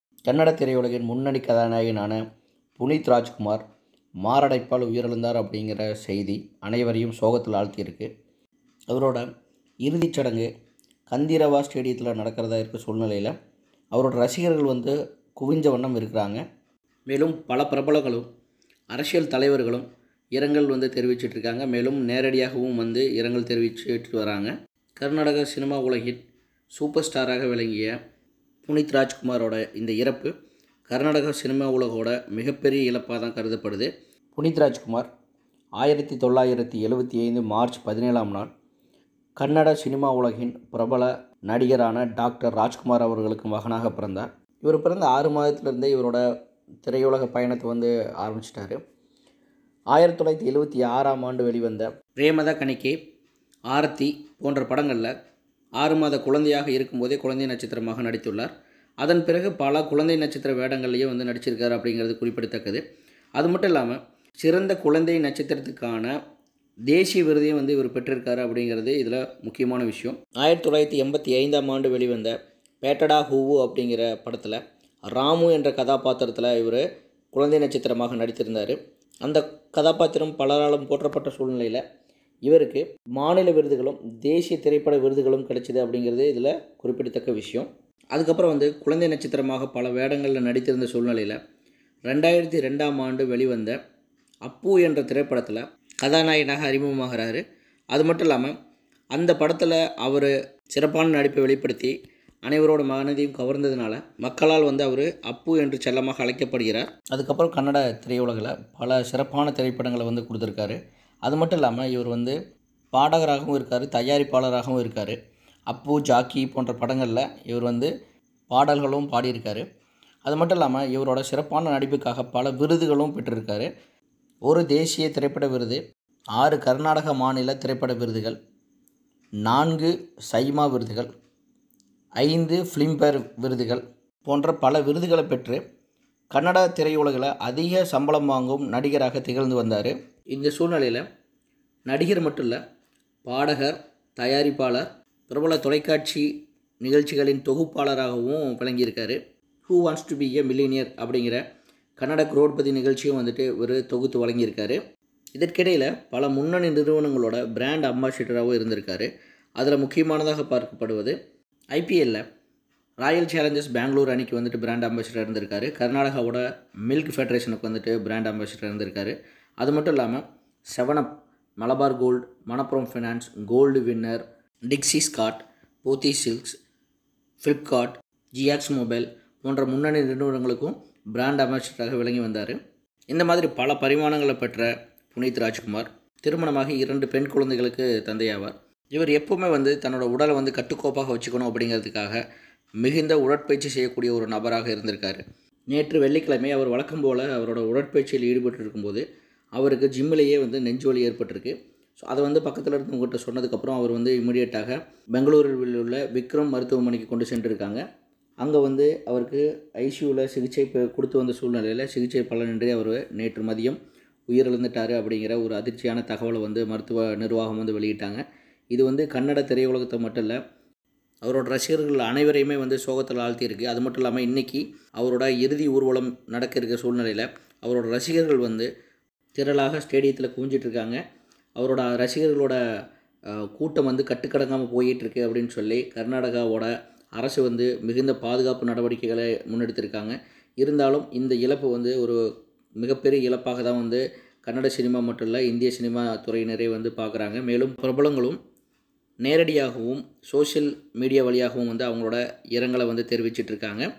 PUNEETH RAJKUMAR Passed Away!!!/பிரபல நடிகருக்கு 46 வயதில் ஏற்பட்ட சோகம்!!!
PUNEETH RAJKUMAR Passed Away!!!/ கன்னட பிரபல நடிகருக்கு ஏற்பட்ட சோகம்!!!